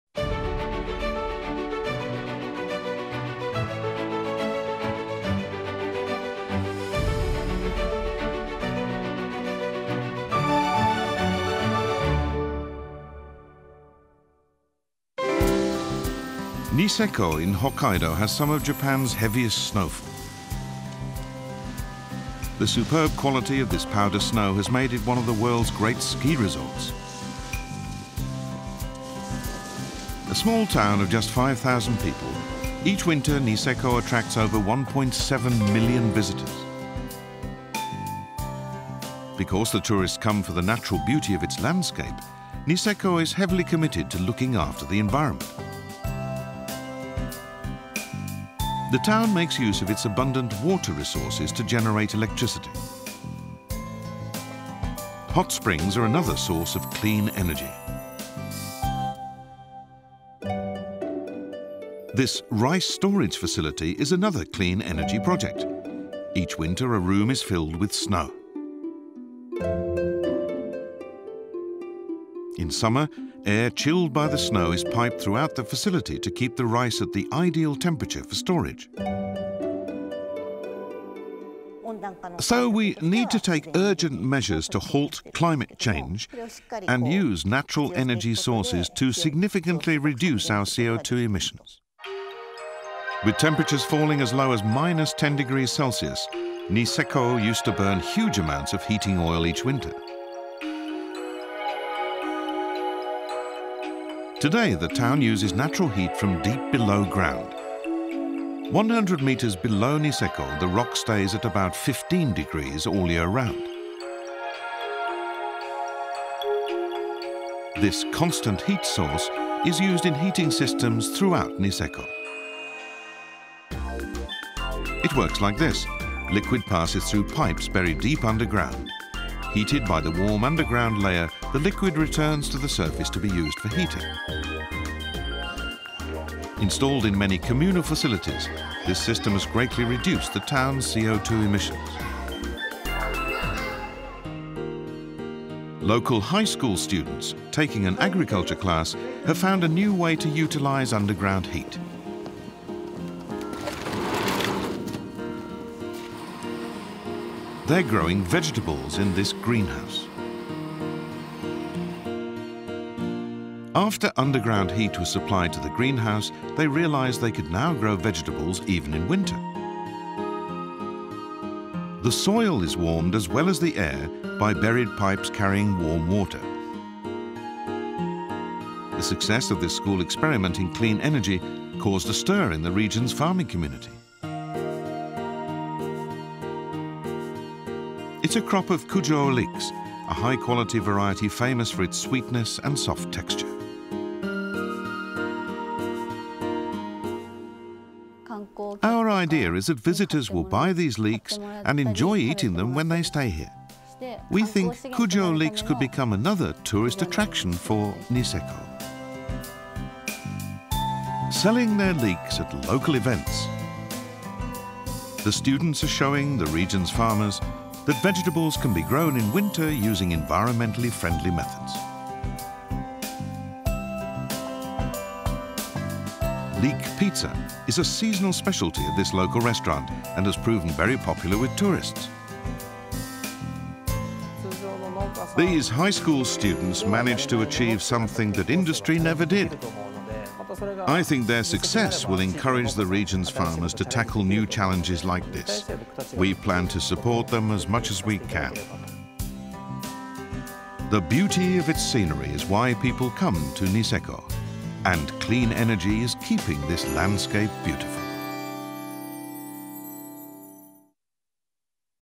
Clean Energy in Snowy Niseko
Niseko, in Hokkaido, has some of Japan’s heaviest snowfall, with great powder snow that attracts over 1.7 million visitors a year. Niseko protects its environment through clean energy initiatives like snow for refrigeration, hot springs for heating, and systems utilizing underground heat. Although it falls to -10°C in winter here, deep underground it stays 15°C year round. Heating systems based on this principle have greatly reduced the town’s CO2 emissions. In another initiative, local high school students are experimenting with using underground heat to grow winter vegetables.